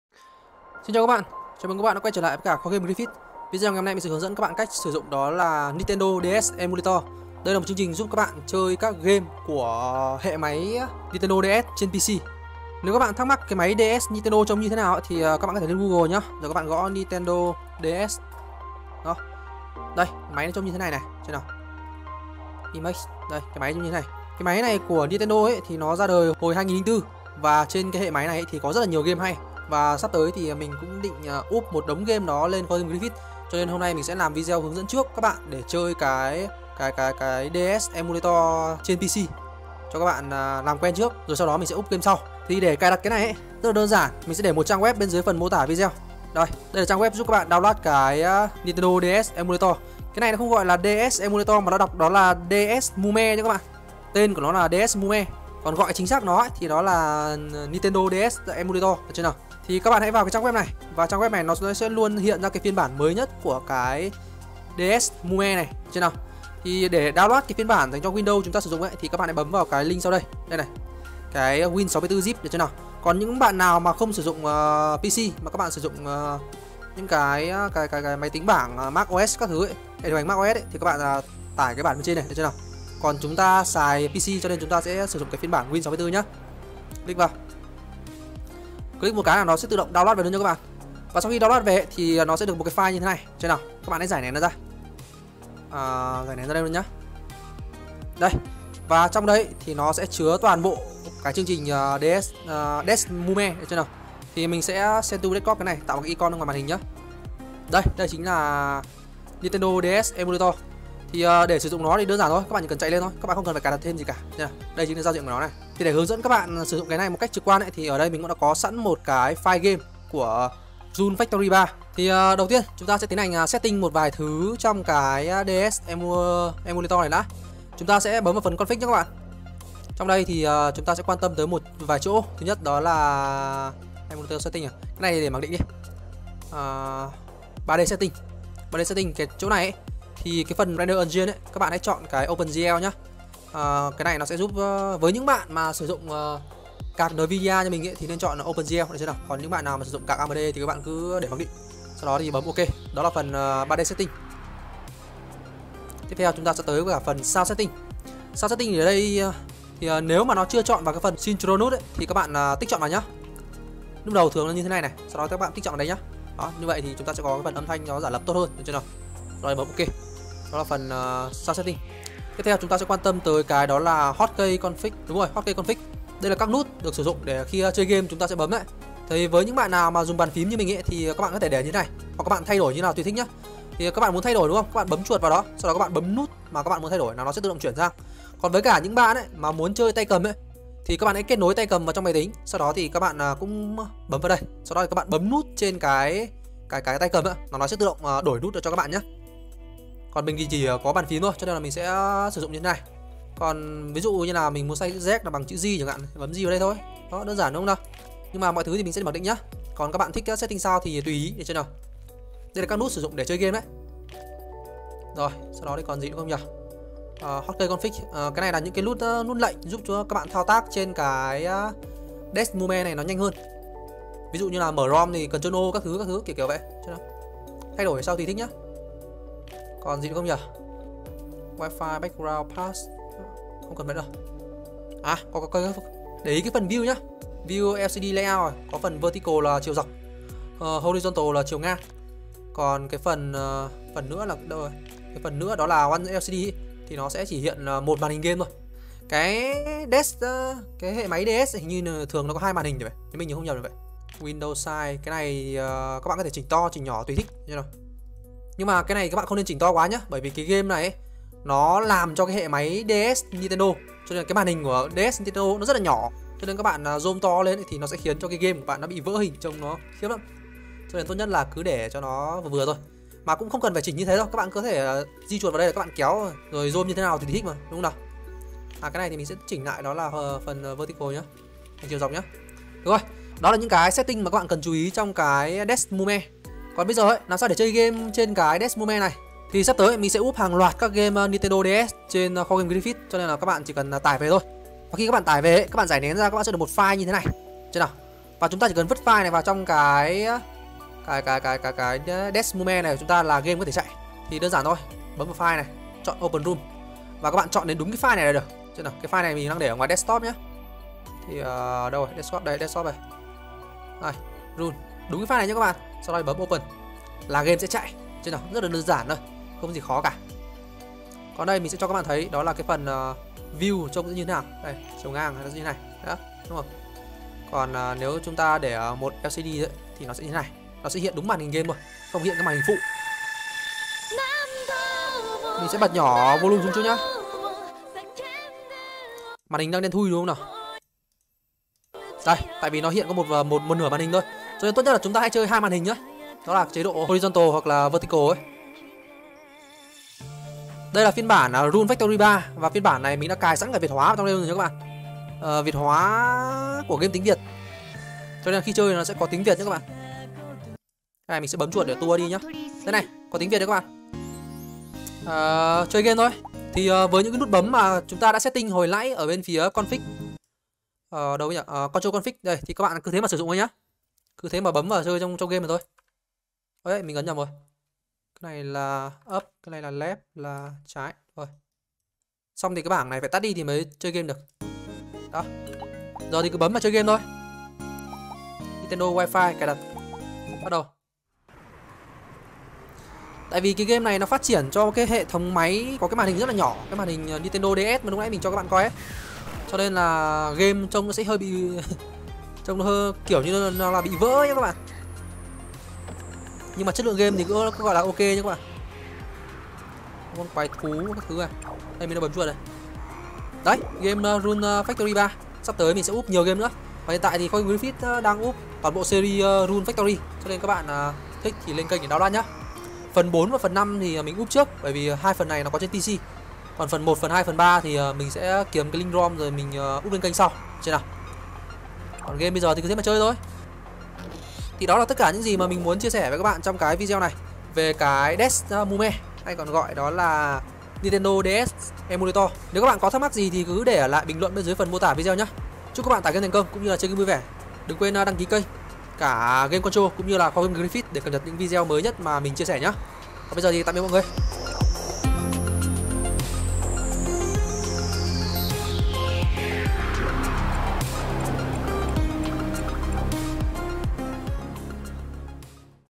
Hướng dẫn sử dụng Nintendo DS Emulator - DeSmuME | Kho GAme Griffith
DeSmuME là ứng dụng 1 giả lập của dòng máy handheld Nintendo DS do công ty Nintendo sản xuất. Video này mình sẽ hướng dẫn các bạn cách sử dụng Nintendo DS Emulator - DeSmuME, để chơi các game giả lập của Nintendo DS trên PC.

MoMo: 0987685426 - Le Tuan Vu
BIDV: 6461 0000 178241 - Le Tuan Vu